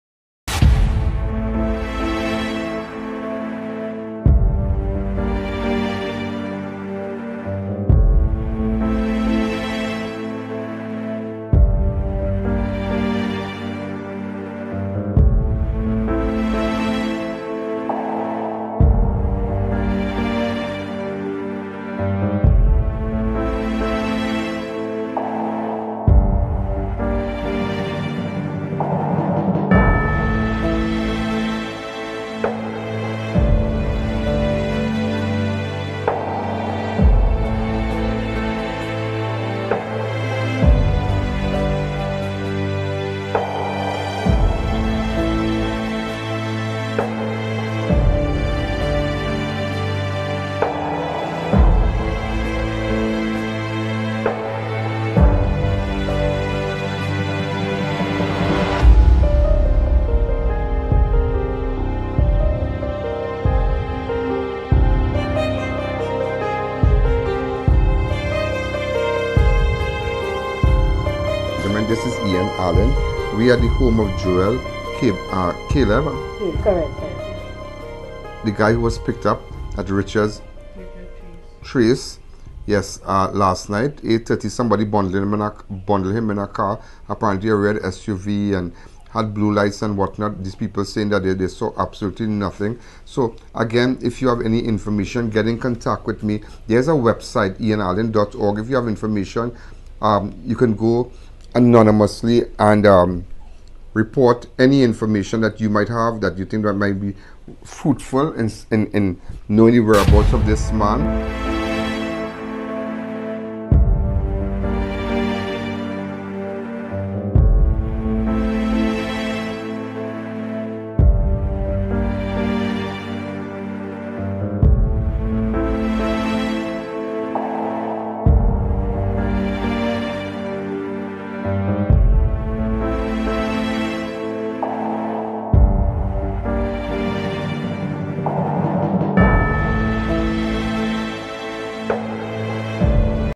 Shocking Discovery in Arima: A Headless Body Found
Almost three days after a man from Arima was abducted outside his home by a group of men impersonating police officers, his headless body was found in a precipice in the Heights of Guanapo, Arima, yesterday morning.

According to reports, early yesterday morning, residents of the community began smelling a strong decomposing smell.

The residents initially suspected that the smell was that of a dead dog that was dumped in the community, but made the grisly discovery while searching a forested area.

According to reports, around 8.30 pm on Wednesday, Caleb was driving his silver Nissan Almera near his home at Tumpuna Road, Arima, when he was intercepted by a red Nissan X-Trail which had blue swivel lights on the dashboard.

Three armed men got out of the SUV, bundled Caleb inside, and drove off with him as a fourth man drove away in Caleb's car.

Caleb's murder raised the count for the year to 196, compared to 173 for the same period, last year.

Detectives of the Region Two Homicide Bureau are continuing investigations

SOCIAL

📷 tnanp868  Add For Graphic Content